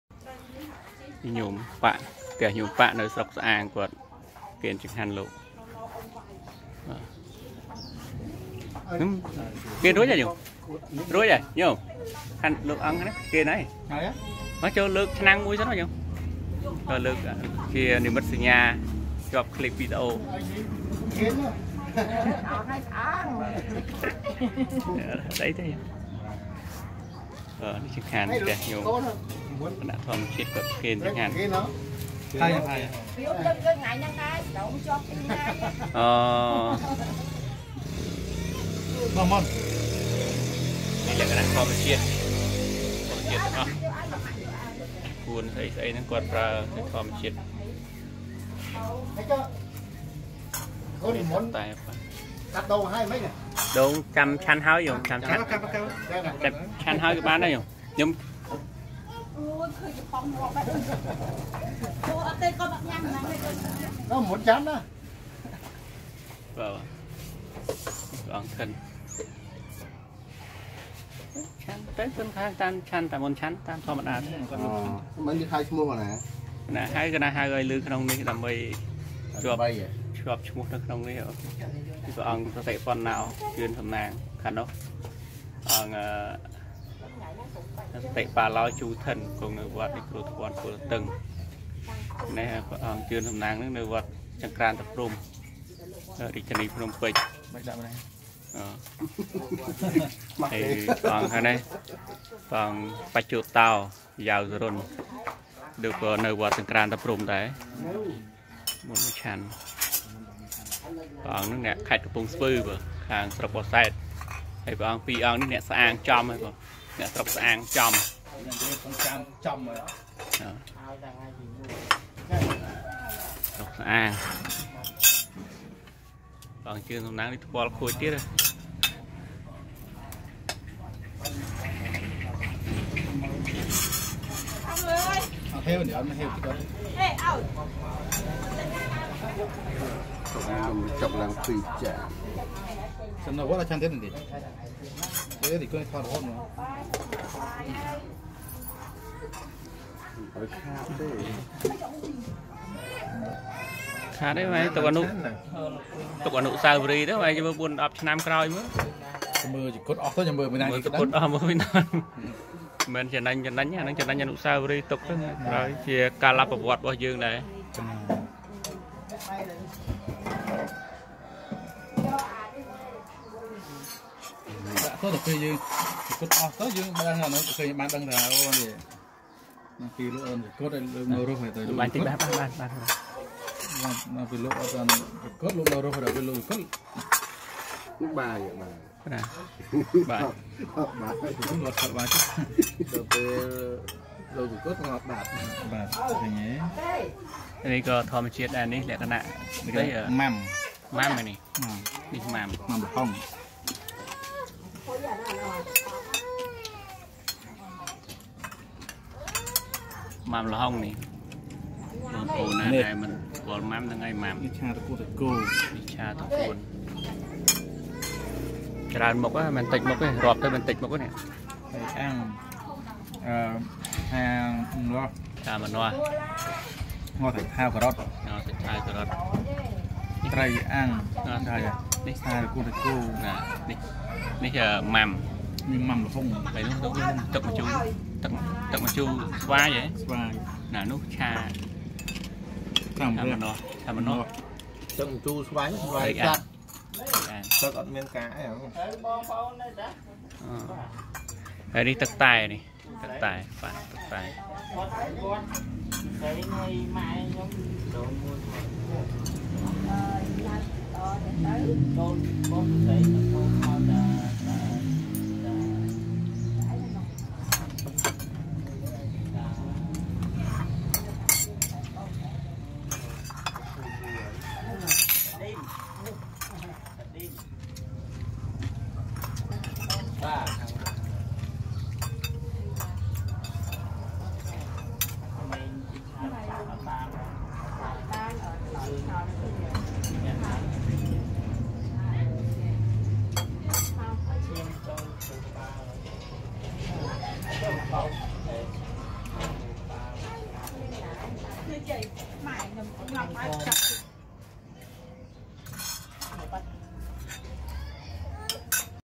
ទទួលចង្ហាន់ត្រង់នៅតាលន់ ស្រុកស្អាង ខេត្តកណ្តាល | IENG SARATH CAMBODIA 🇰🇭
ទទួលចង្ហាន់ត្រង់នៅផ្ទះឧបាសិកា ប៉ាក់ ភូមិតាលន់ ឃុំតាលន់ ស្រុកស្អាង ខេត្តកណ្តាល។